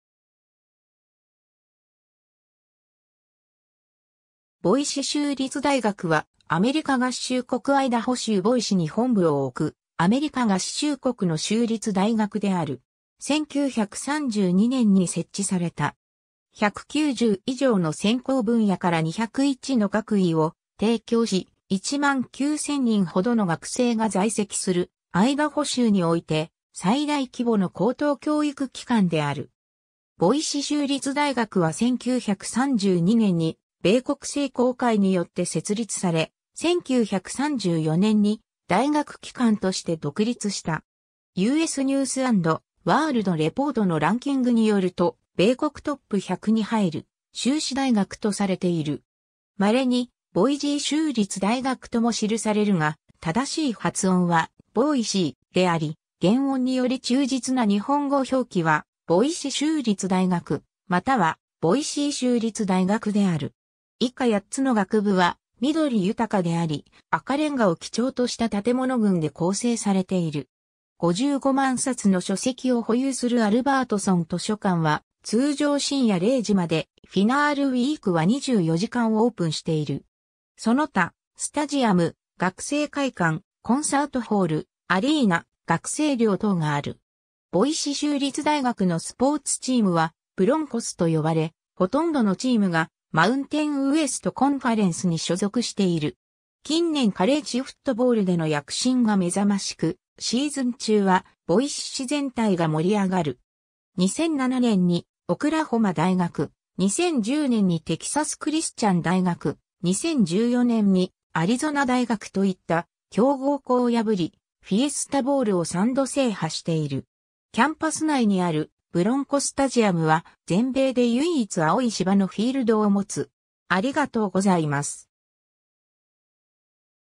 ボイシ州立大学
ボイシ州立大学（英語: Boise State University）は、アメリカ合衆国アイダホ州ボイシに本部を置くアメリカ合衆国の州立大学である。
1932年に設置された。
 190以上の専攻分野から201の学位を提供し、19,000人程の学生が在籍する、アイダホ州において最大規模の高等教育機関である。
ボイシ州立大学は1932年に米国聖公会によって設立され、1934年に大学機関として独立した。
U.S. News & World Reportのランキングによると米国トップ100に入る修士大学とされている。
稀にボイジー州立大学とも記されるが、正しい発音は"BOY-see"、であり原音により忠実な日本語表記はボイシ州立大学、またはボイシー州立大学である。
 以下8つの学部（College)によって組織されている。 本部キャンパスは、ボイシ川を挟みジュリア・デイヴィス公園の正面に位置する。ボイシ空港からバスで10分、ダウンタウンへは徒歩圏内であり、アクセスに優れている。広大なキャンパス（708,000 m²）は、緑豊かであり、赤レンガを基調とした建物群で構成されている。
55万冊の書籍を保有するアルバートソン図書館は、通常深夜0時まで、Final Week（試験期間）は24時間オープンしている。
その他スタジアム、学生会館、コンサートホール、アリーナ、学生寮等がある。
 ボイシ州立大学のスポーツ・チームは、ブロンコス(Broncos)と呼ばれ、ほとんどのチームがMountain West Conference(MW:NCAAのFBSに属するカンファレンス)に所属している。
近年カレッジフットボールでの躍進が目覚ましく、シーズン中はボイシ市全体が盛り上がる。
2007年にオクラホマ大学、2010年にテキサスクリスチャン大学､2014年にアリゾナ大学といった強豪校を破り、フィエスタボウルを3度制覇している。
キャンパス内にあるブロンコ・スタジアム（収容人数:36,387人）は全米で唯一青い芝のフィールドを持つ。